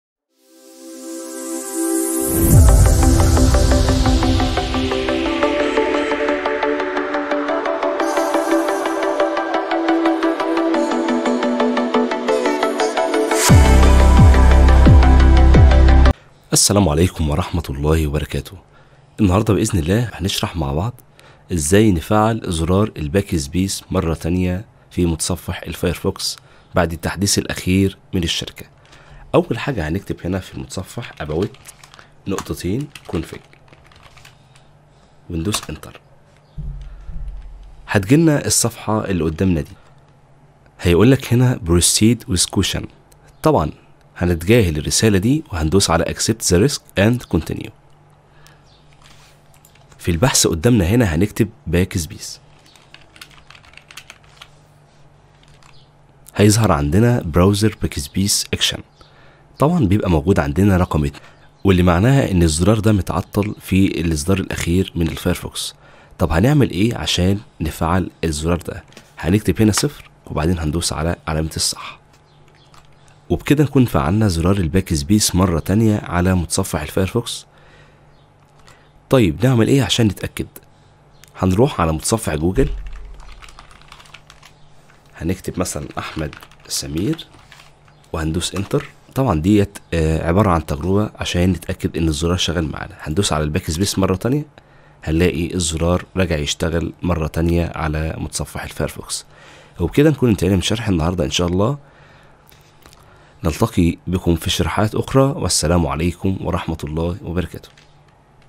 How to restore Backspace button to work in Firefox again [ Firefox 86 ]
How to restore Backspace to Firefox again [ Firefox 86 ]

In the near future Mozilla will disable the history back navigation with the Backspace key. Currently, if you hit the Backspace key, you will directly go to a previous page in the browser history. This will no longer works starting in Firefox 86.

Now , You can't use the backspace again in firefox 86 but we will help you in this video to get this button back to work without any issues.

Firefox browser, Firefox update, Mozilla Firefox, Google Chrome